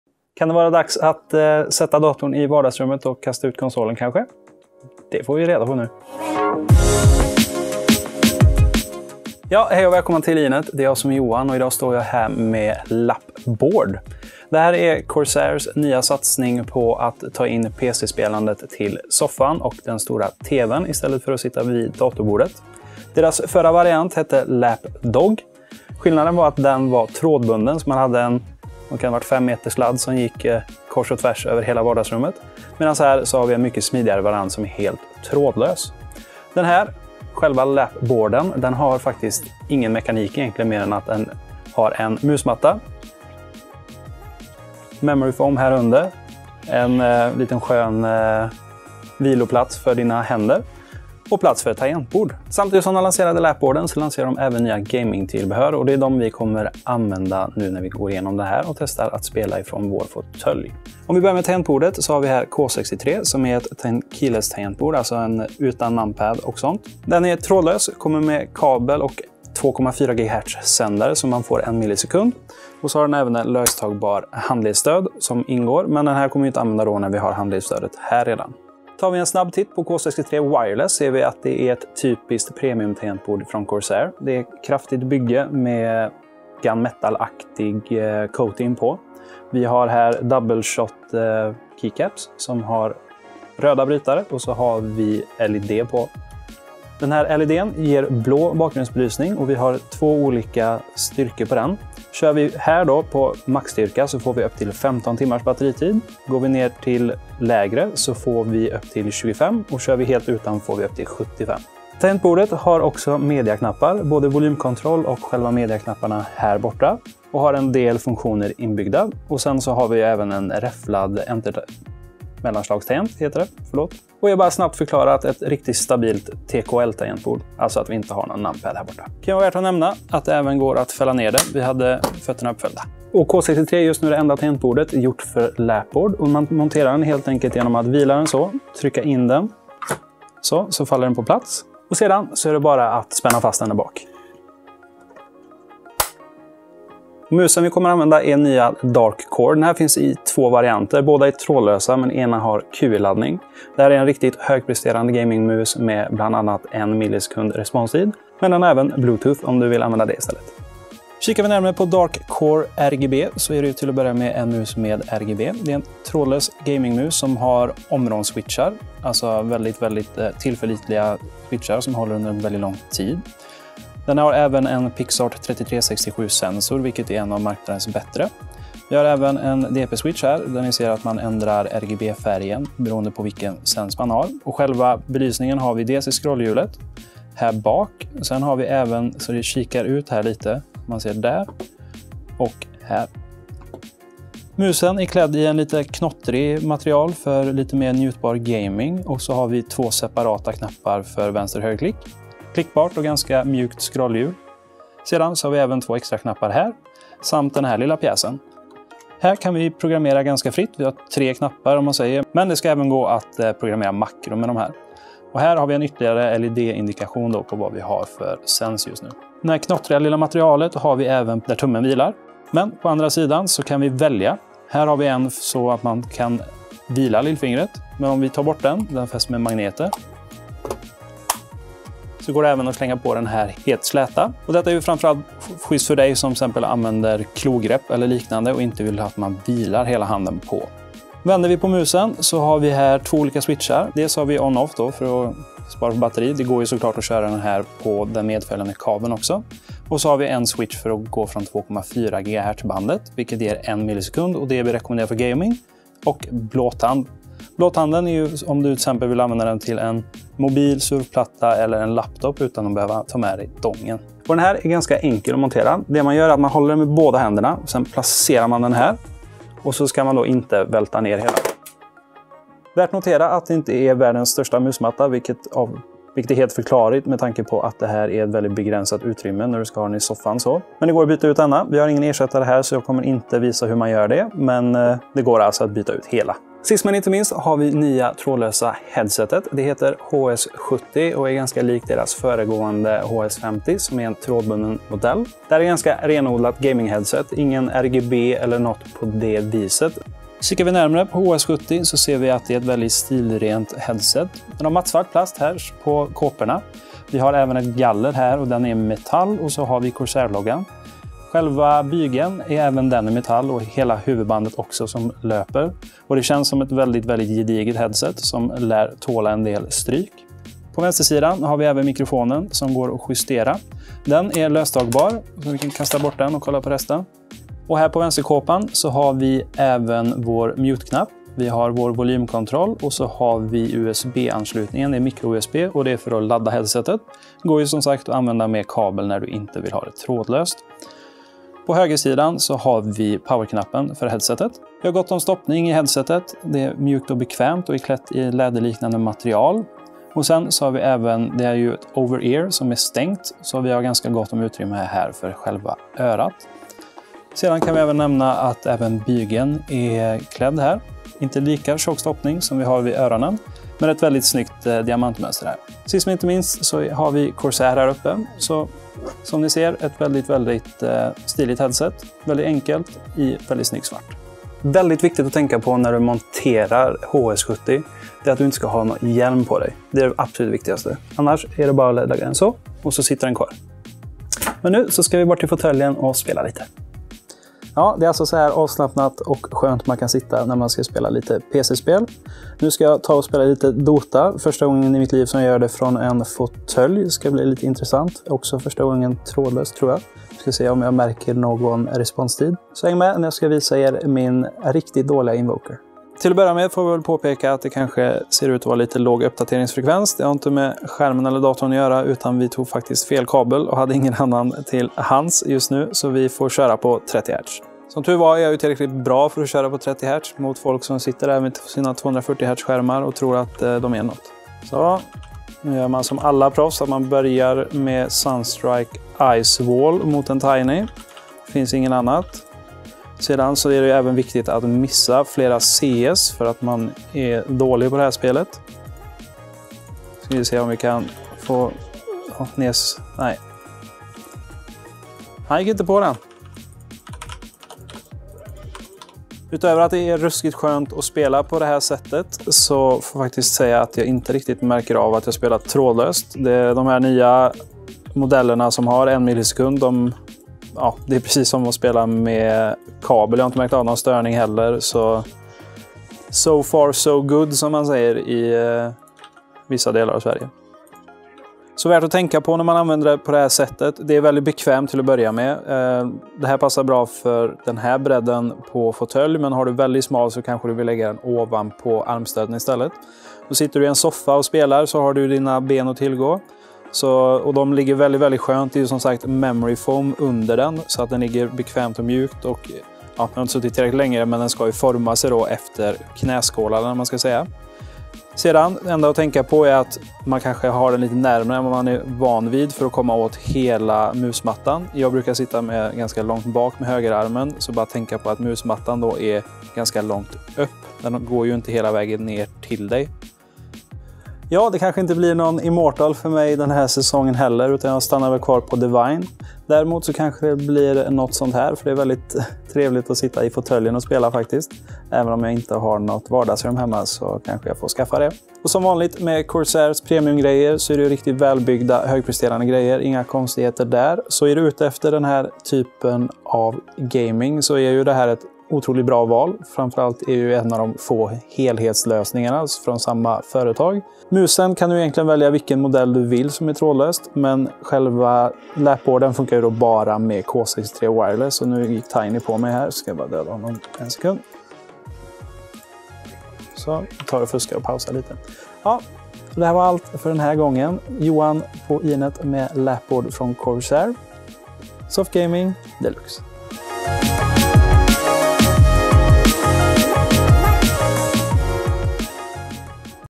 Mus och tangentbord i soffan? Vi provar Corsair Lapboard!
Är du sugen på att spela på datorn bakåtlutad i soffan? Läs då mer om produkterna vi gick igenom i videon nedan!